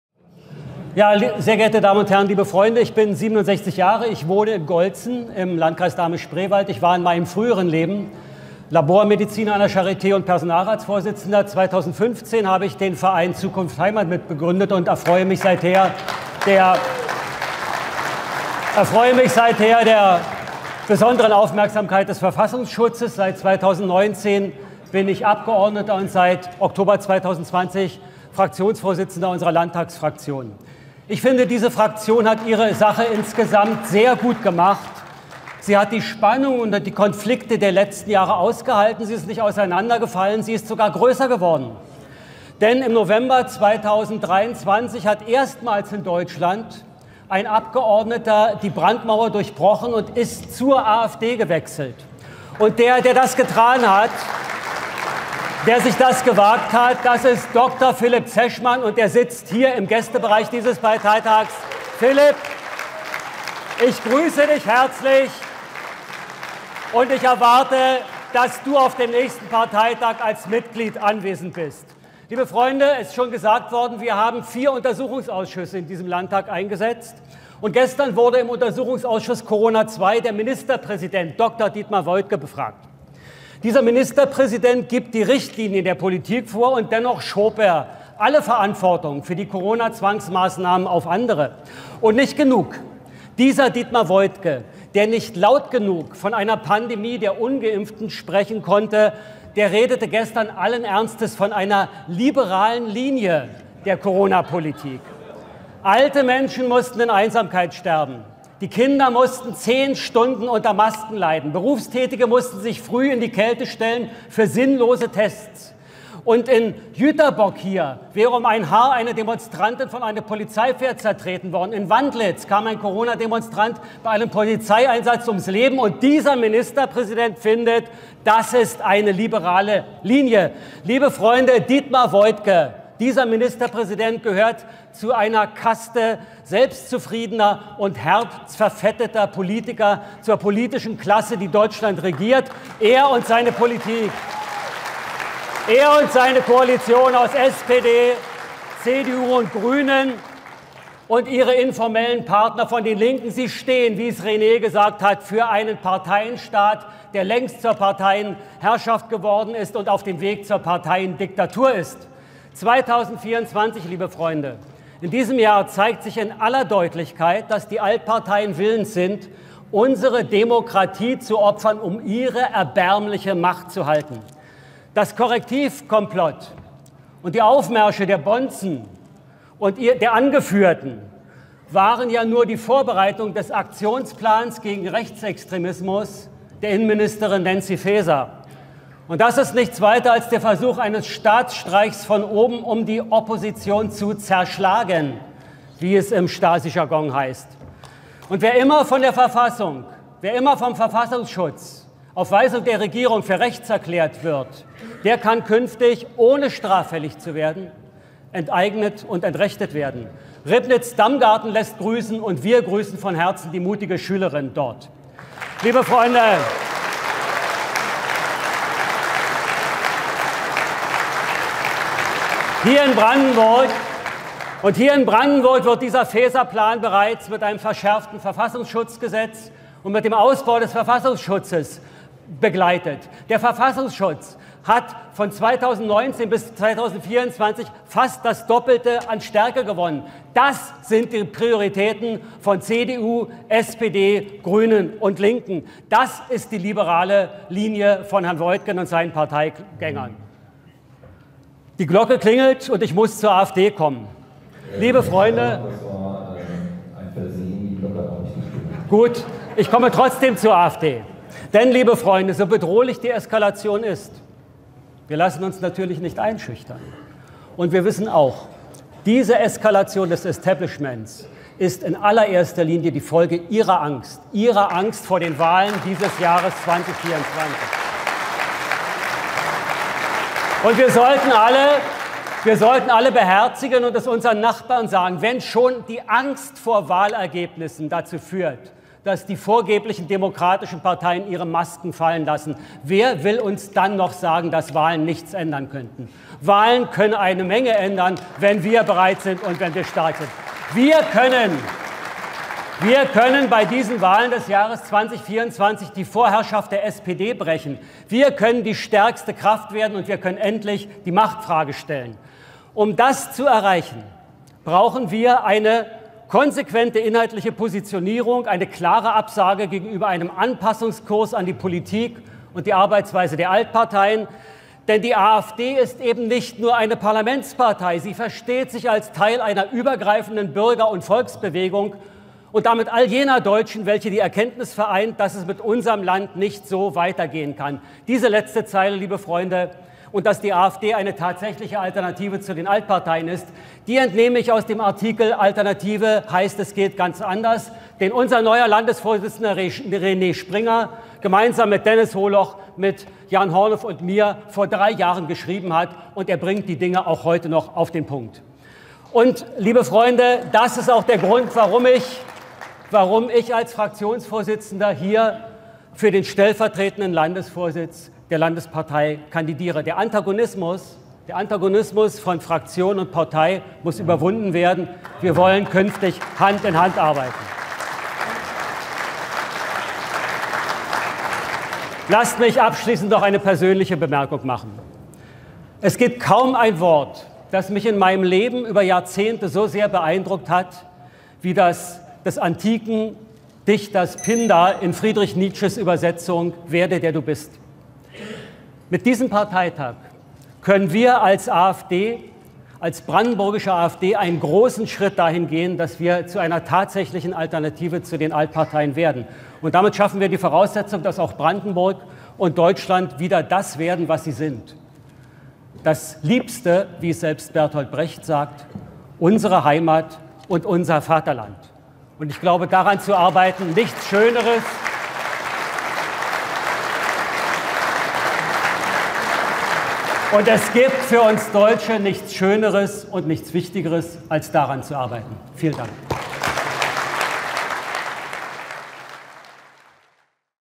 Landesparteitag Jüterbog | 16. März 2024 | Dr. Christoph Berndt
Meine Rede für die Wahl zum zweiten stellvertretenden Landesvorsitzenden der AfD Brandenburg.